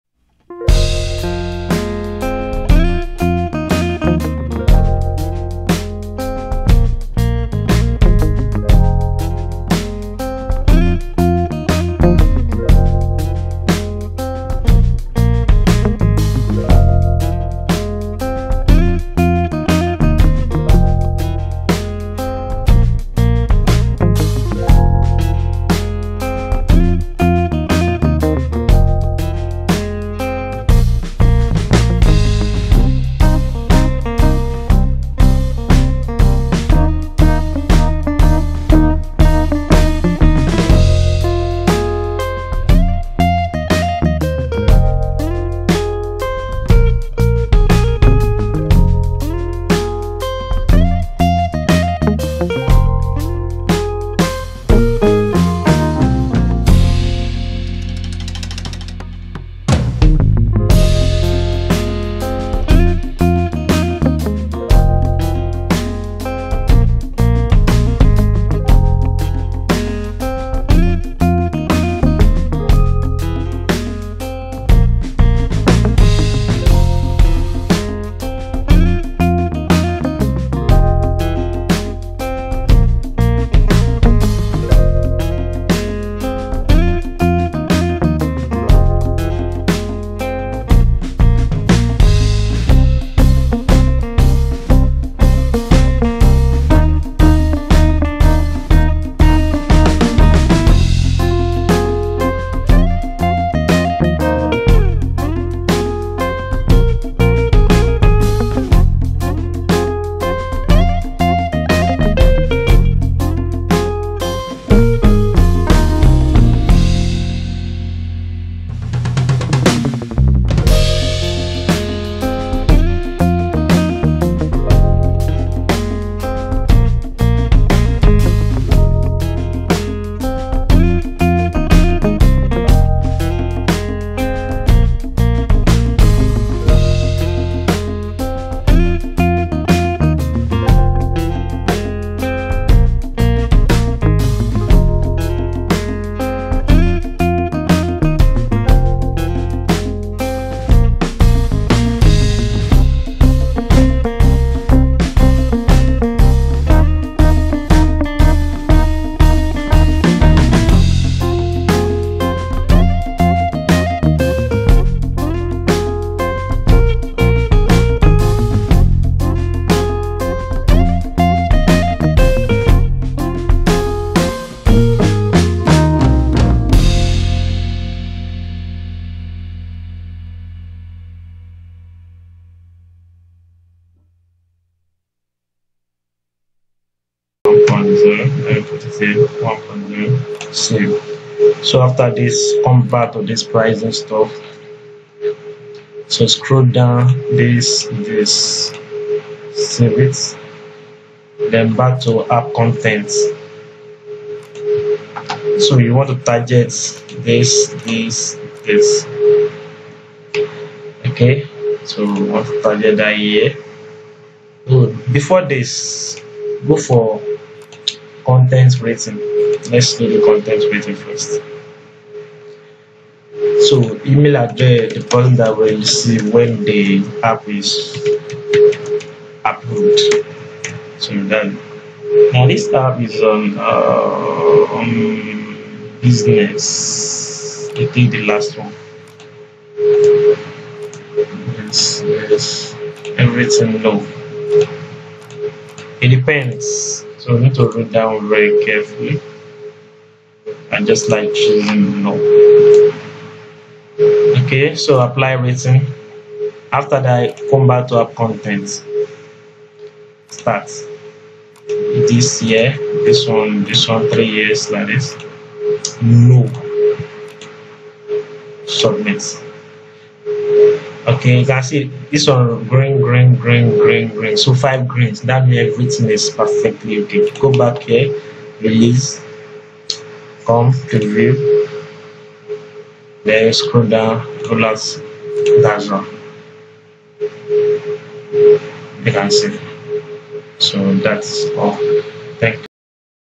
How To Upload Android App on Google Play Store | Publish App on Play Store
Requirement To Publish App on Google Play Store
- Have Google Play Console Account ( Registration Fee $25 onetime )
- Game or App APK File
- App Icon : 512x512
- Apps ScreenShot : 2 Images
- Featured Image : 1024 w x 500 h

Steps To Publish APP / Game on Google Play Store
- Create Account on Google Play Console :
- Click on Create Application
- Fill out Basic information about APP
- Complete Store Listing
- Upload APK File
- Pricing & Distribution
- App Content Rating
- App Content
- Send App for Review
- Done

App / Game Review Process Time Takes up to 48 hours
once our APP Approved, it will publish on google play store